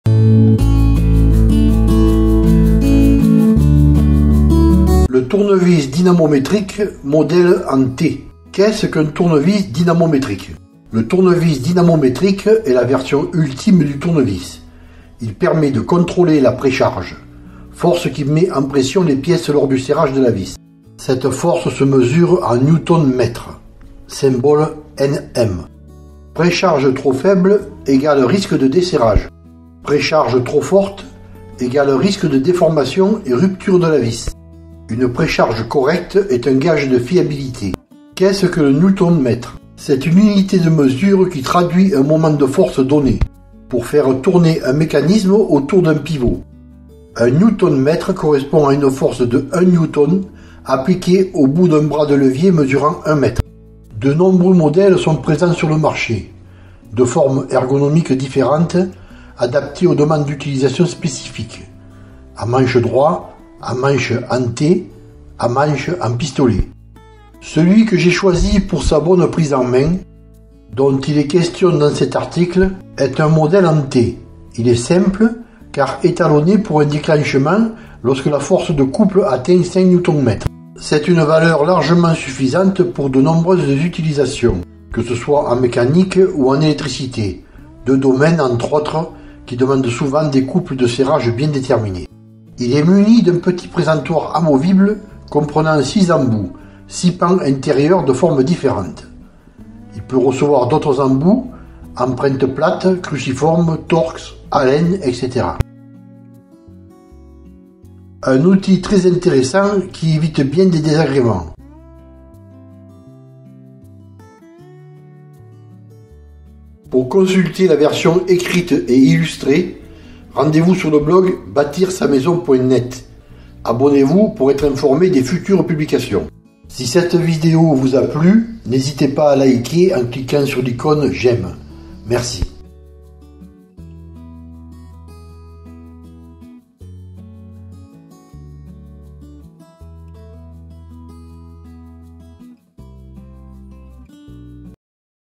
Le Tournevis Dynamométrique modèle en T
Vidéo : Le Tournevis Dynamométrique modèle en T
Dans cette vidéo je vous présente un outil très utile dans de nombreux actes, que ce soit en mécanique, électricité, ou tout autres domaines nécessitant des valeurs de serrage précises.
Pour consulter la version écrite et illustrée rendez vous sur le blog :
 « j’aime »  Merci !